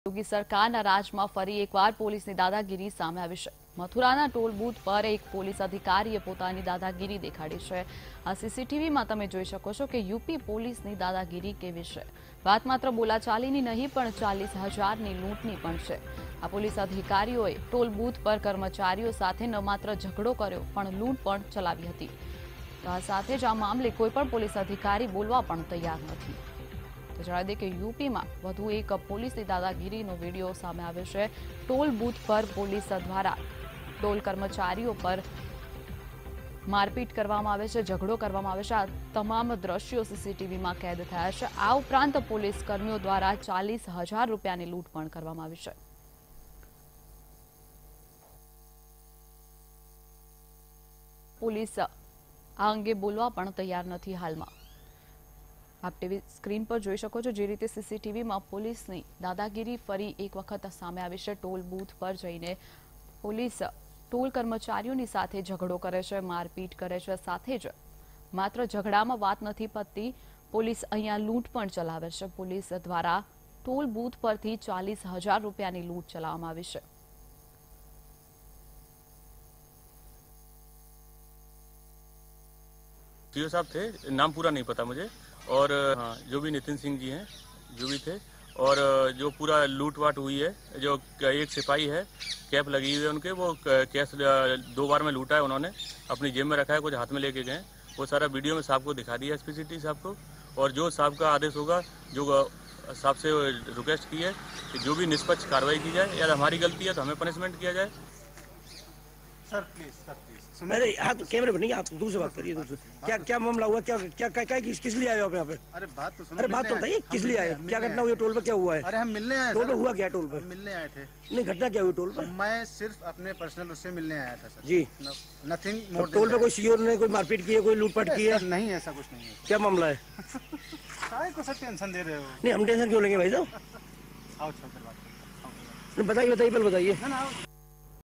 Mathura : Dabang Police men ruthlessly thrashes toll booth workers, also looted Rs. 40K | Vtv News
Mathura : Dabang Police men ruthlessly thrashes toll booth workers, also looted Rs. 40K .